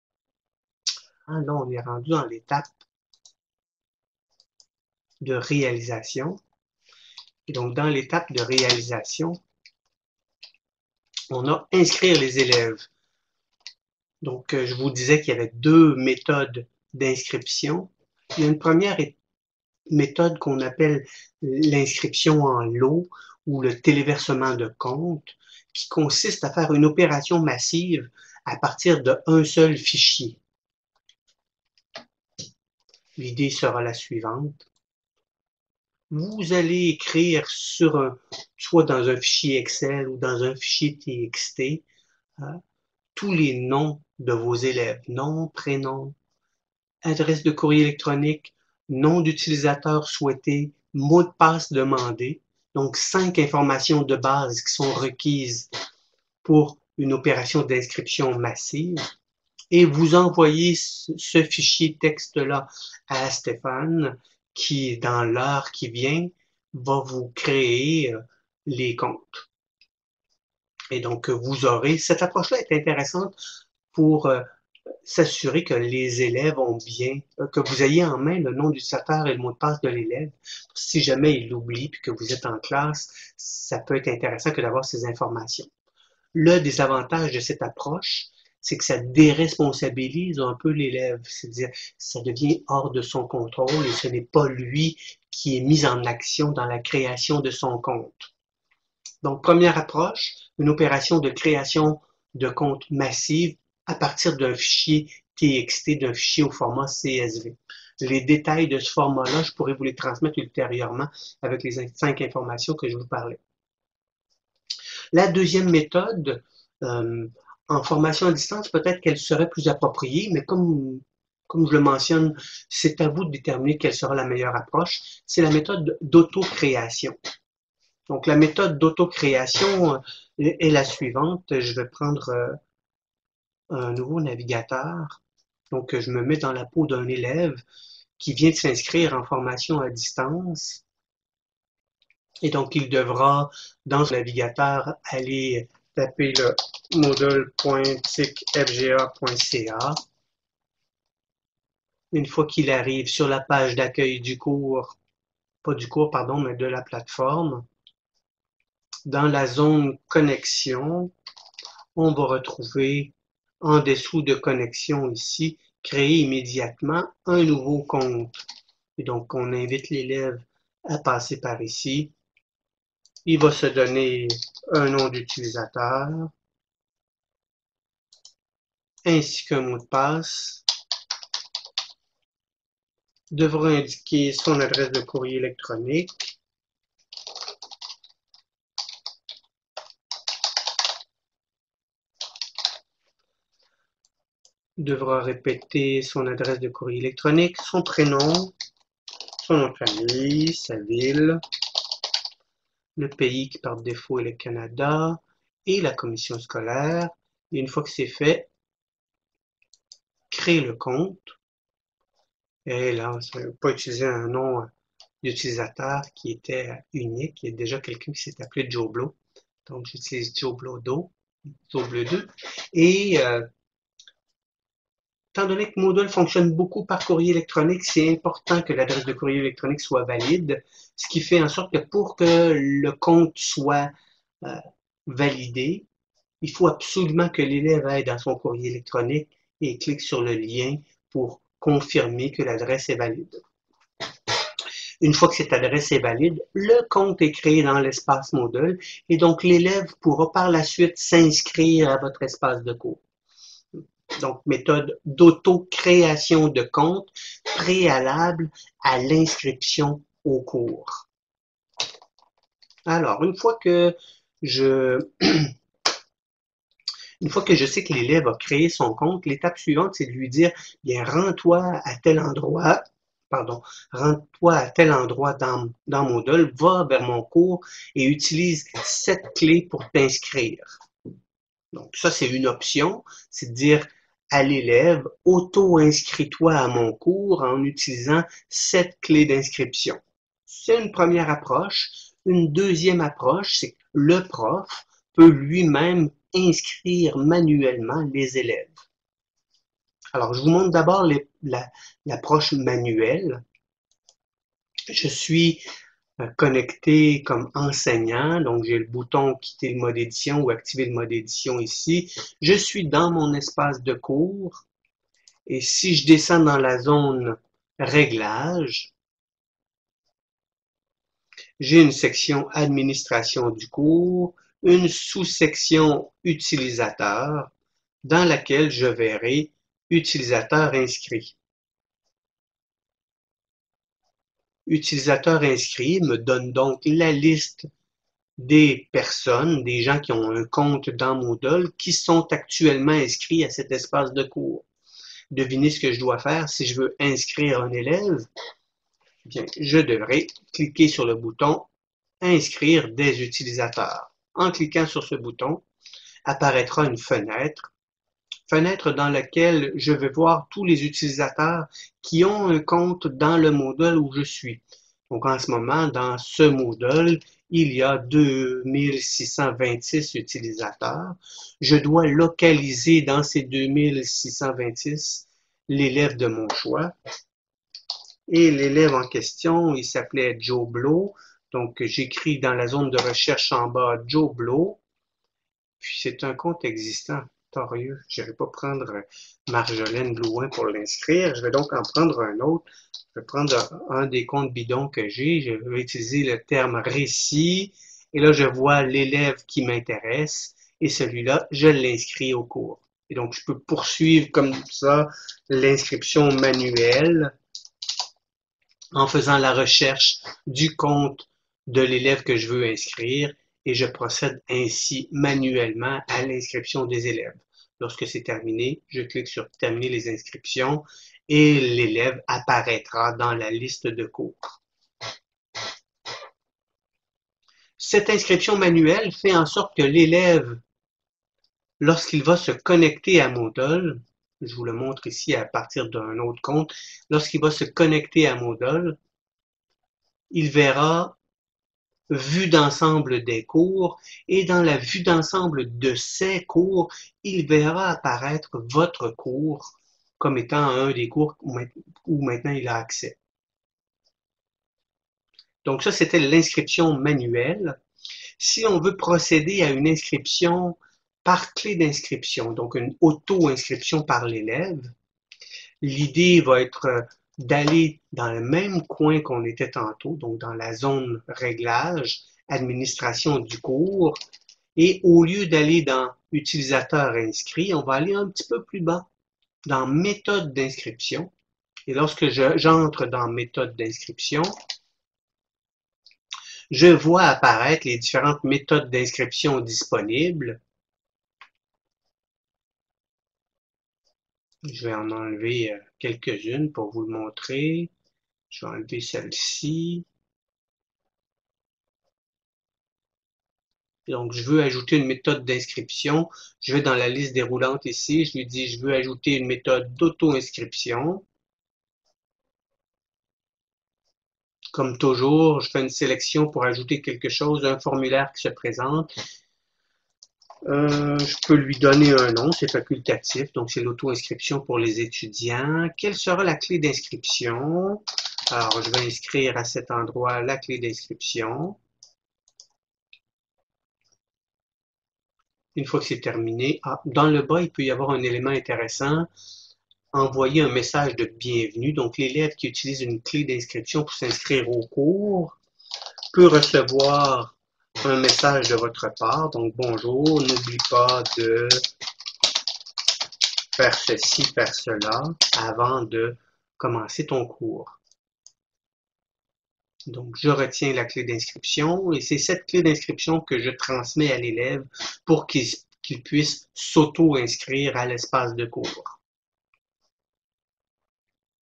Inscrire des élèves dans Moodle
Inscription en lot et auto-inscription.